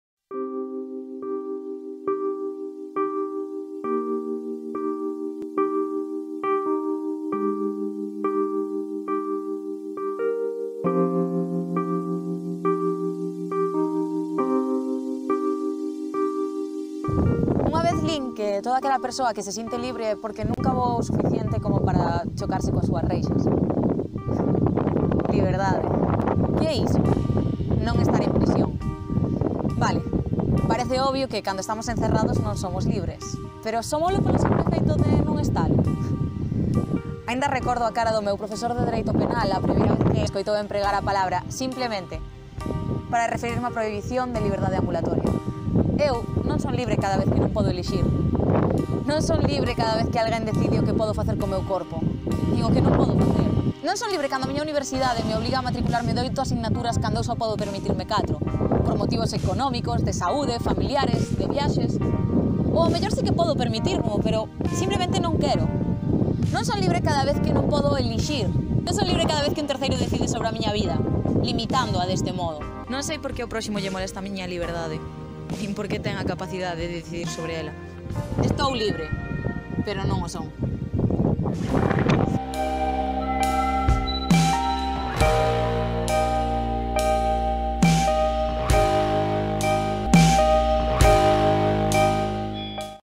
LIBERDADE: ¿que é iso?
Preséntovos un dos meus textos máis sentidos e unha das miñas frustracións máis recurrentes. Oxalá algún día o próximo me permita abrazar a miña maior musa como me gustaría.

*"Privación" é a palabra que ten que acompañar a "liberdade ambulatoria", non "prohibición". Ese desliz debería ser suficiente para negarme o título de xurista.

¡GRACIAS POR ESCOITARME!